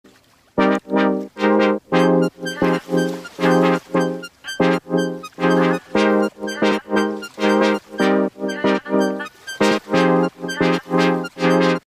swimming cats
in this video you can see swimming cats
cats jump into water and swim
cute cats jump into water and having fun